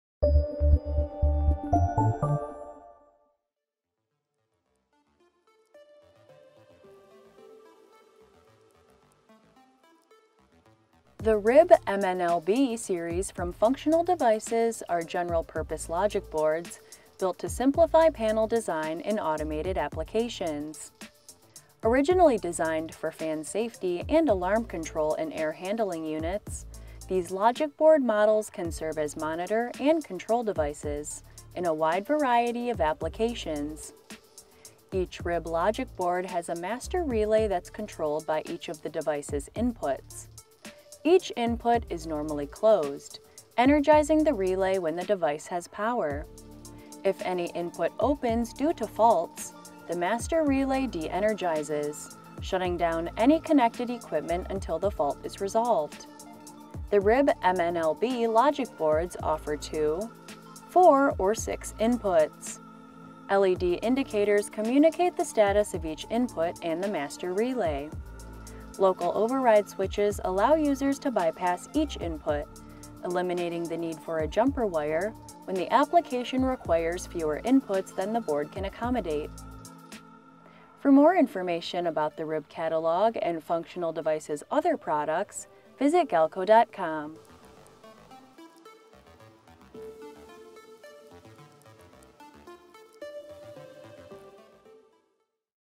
RIB® Functional Devices MNLB Series Logic Boards
The RIB MNLB from Functional Devices are general purpose logic boards built to simplify panel design in automated applications. Originally designed for fan safety and alarm control in air handling units, these logic boards can serve as monitor and control devices in a wide variety of applications.
Each RIB logic board has a master relay that’s controlled by each of the device’s inputs. Each input is normally closed, energizing the relay when the device has power. In any input opens due to faults, the master relay de-energizes, shutting down any connected equipment until the fault is resolved.